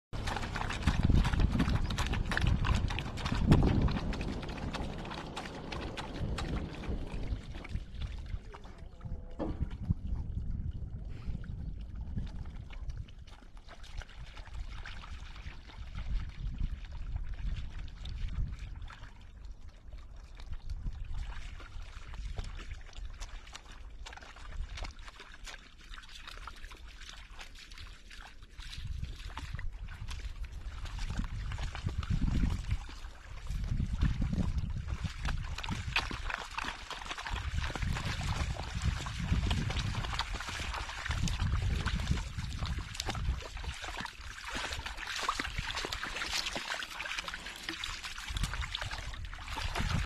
WAFAAMU WANAJESHI WA MBUGANI.
Mbwa mwitu nijamii ya mbwa wanaopatikana polini familia yao ni (canidae) wanaishi kwa magrupu Ndani ya group  Moja wanaweza fika mbwa mwitu 30 , wanawinda kwa ushirikiano mkubwa.